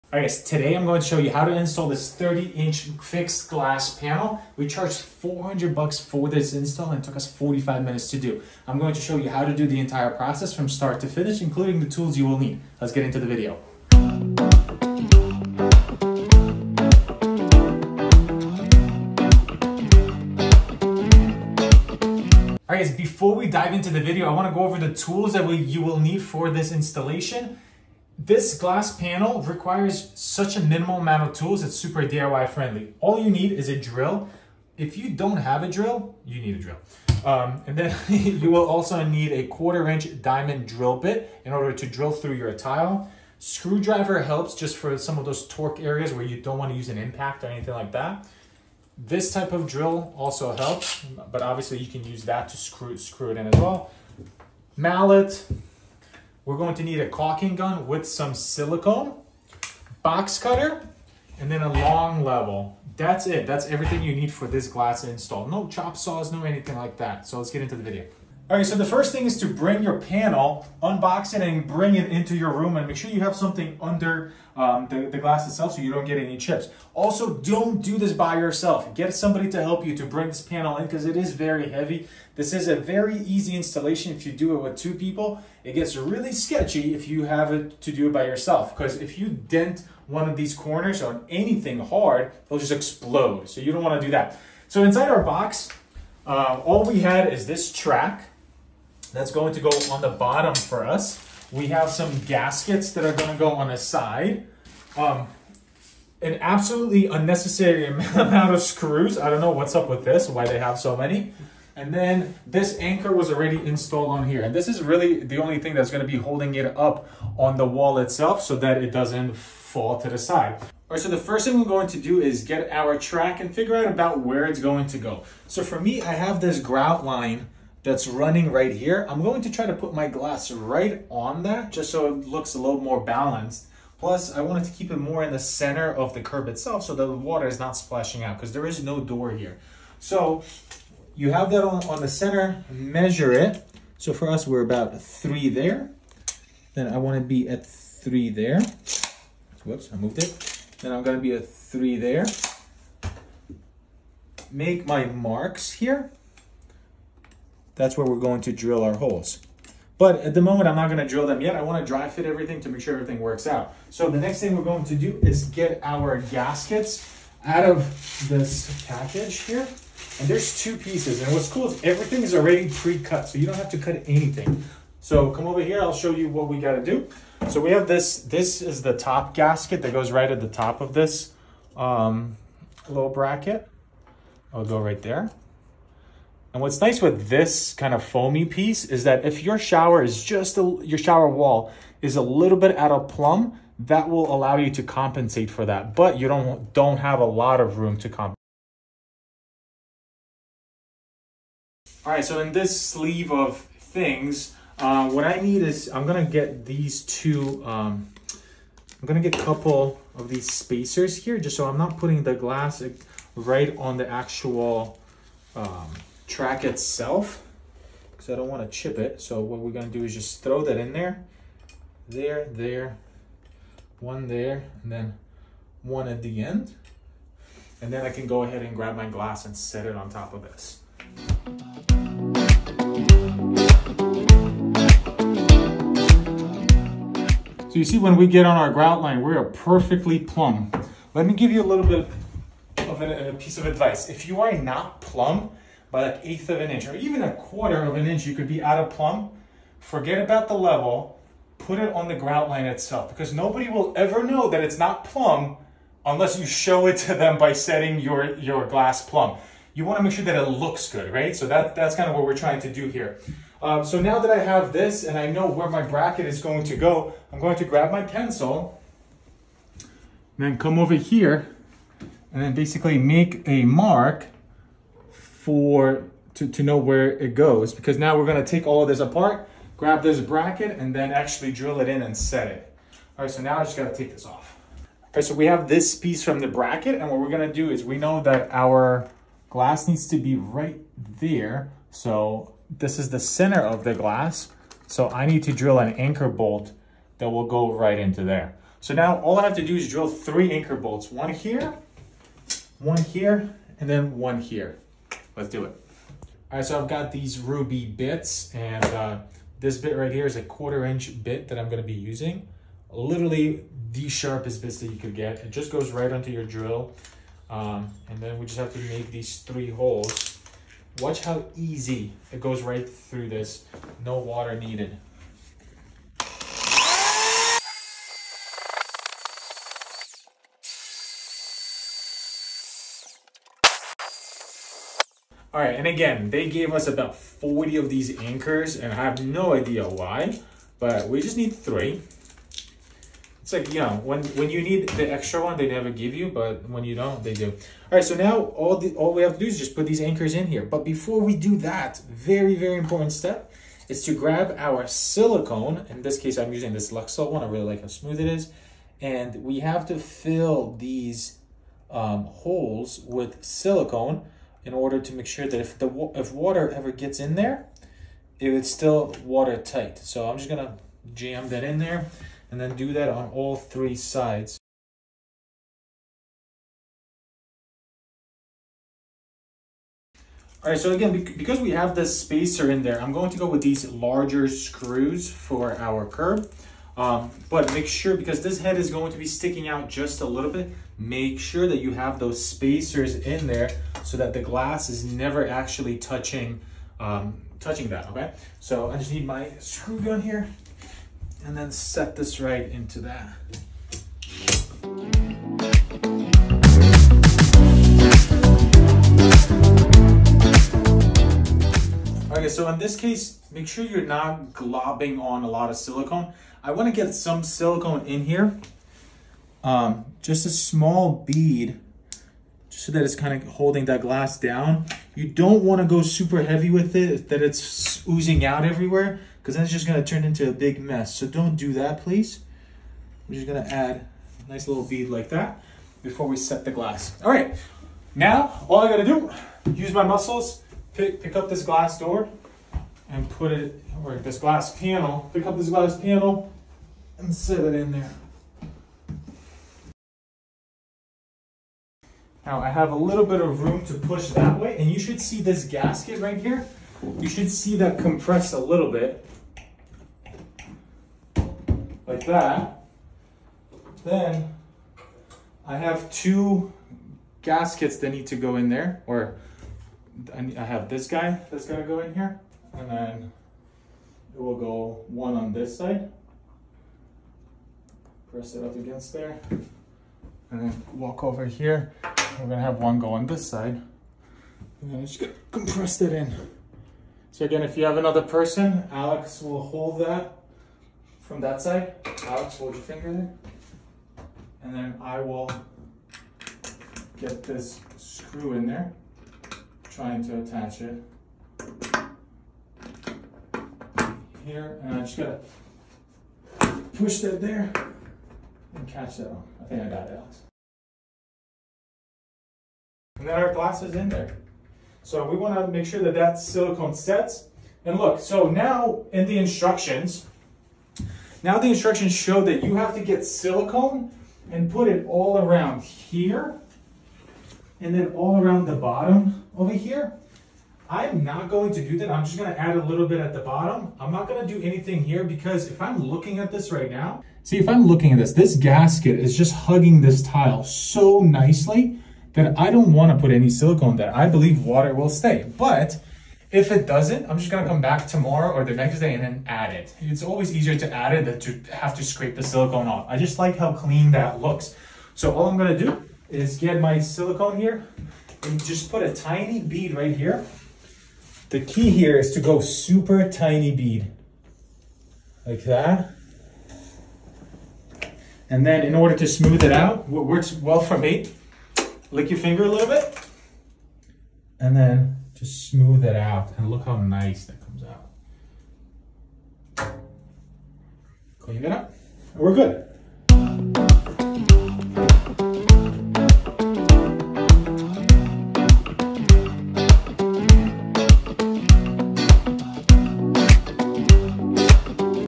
How to install a Shower Glass Panel. EASY DIY. WINNI
Today I am going to show you how to install this 36" shower glass panel. For this work we charged $400 and it took us 45 min to do. While many of you will freak out about the price, just know that Glass company was going to charge $600 for the labor. So we were still under the market price.

This panel that we installed is by far my top 5 best shower panels that i have installed. It was very easy and did not reacquire many tools for the install. It is very DIY friendly project.

WINNI